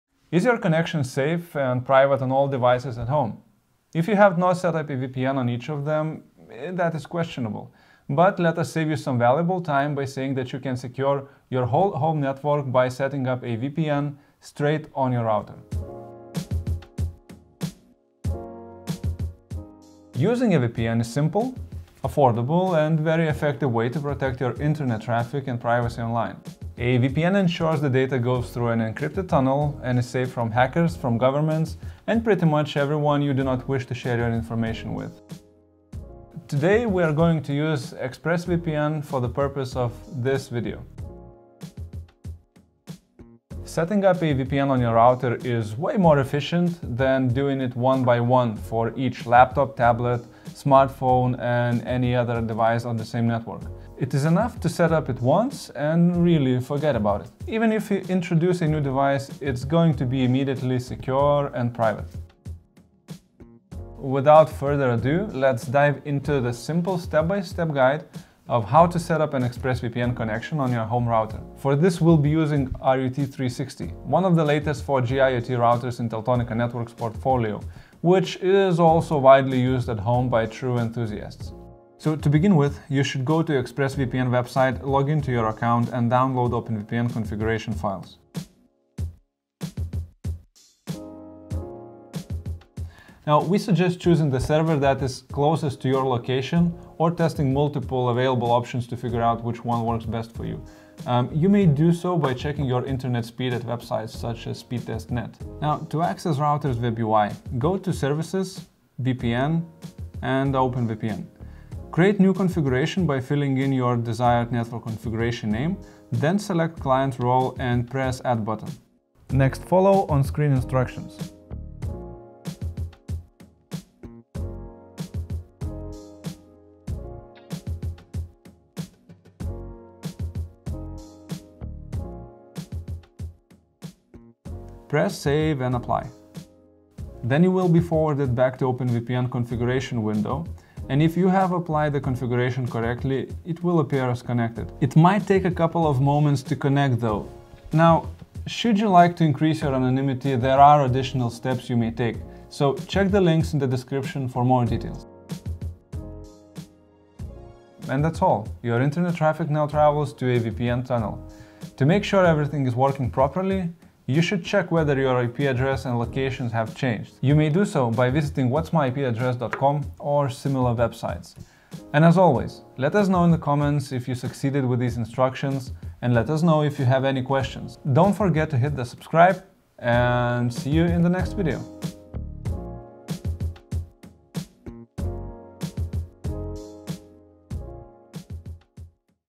How to Set up a VPN Using ExpressVPN & RUT360 Router
Save some valuable time by securing your whole home network by setting up a VPN straight on your router. Watch this simple step-by-step guide to set up an ExpressVPN connection on your home router.

Timestamps:
00:00 Intro
00:22 What is VPN?
00:44 Set up a VPN on my router or separate devices?
1:23 Setup a VPN on Router using ExpressVPN
2:37 Summary
3:01 Outro